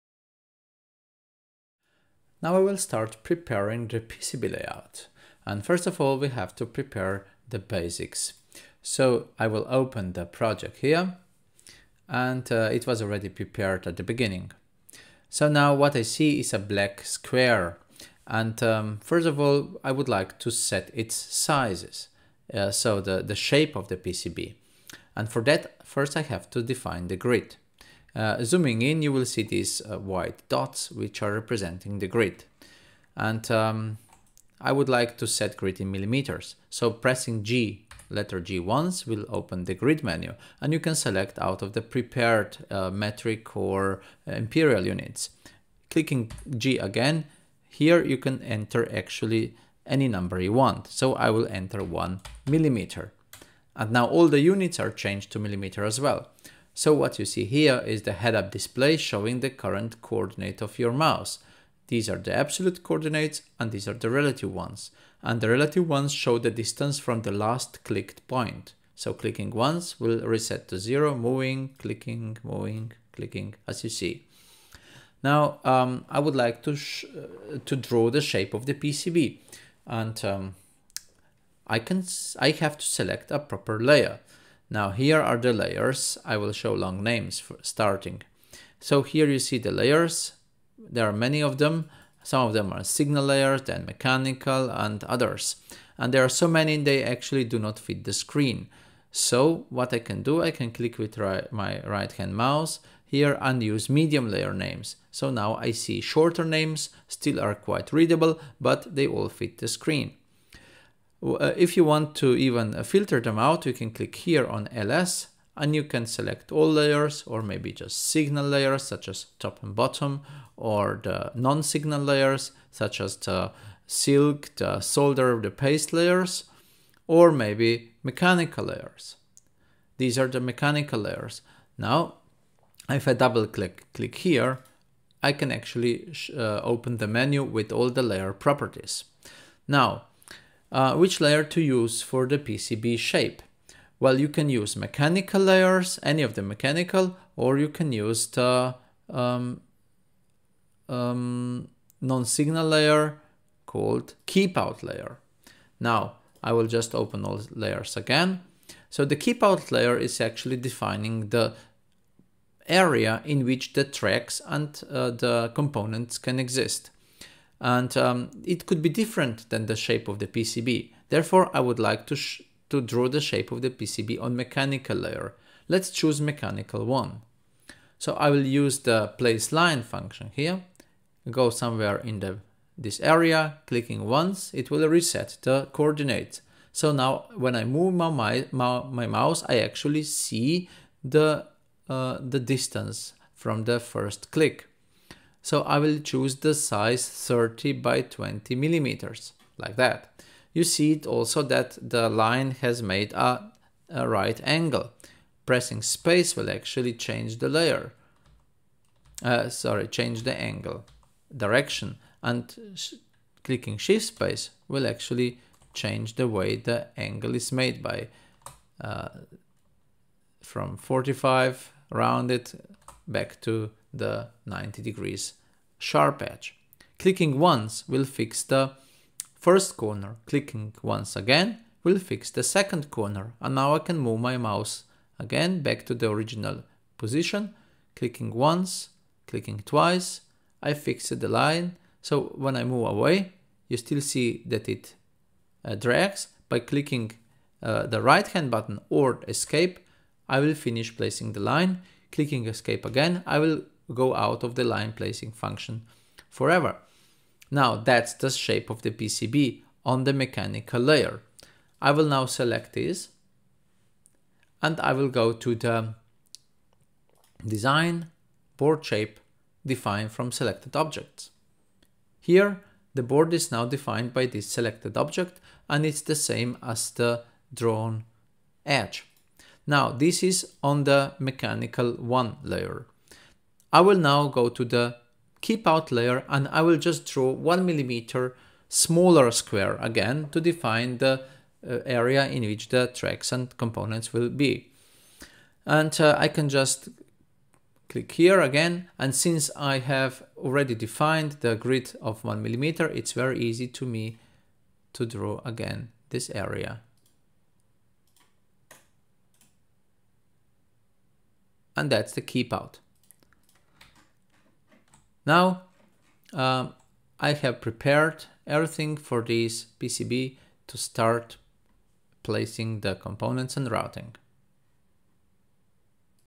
Altium intro #20: Preparing PCB design
Setting the grid, defining pcb shape, layers.